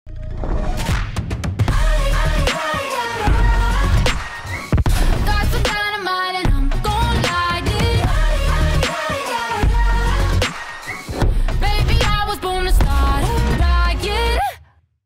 New Perspectives w/ Global Access 🌐 | JD Sports US
Featuring SAINt JHN, Fletcher, & Lil Durk in the latest Global Access styles.